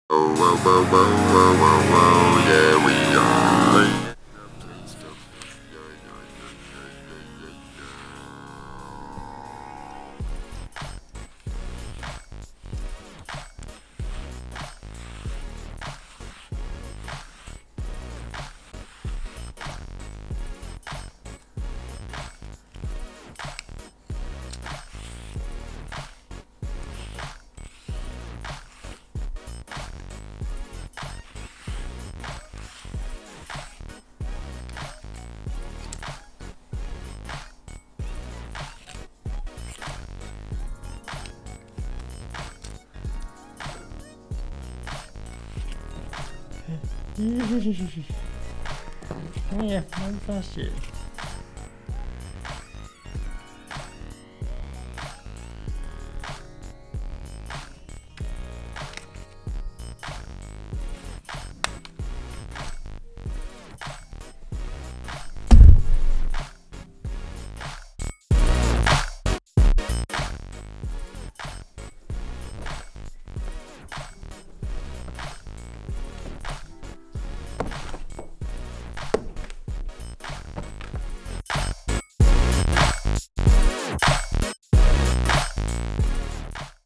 Cat vs Brush. Cat wins.
Our cat Sparky is getting quite old and is not so good at cleaning himself, so needs a regular brush. He likes being brushed, up to a point, and then he starts to get grumpy as you will see.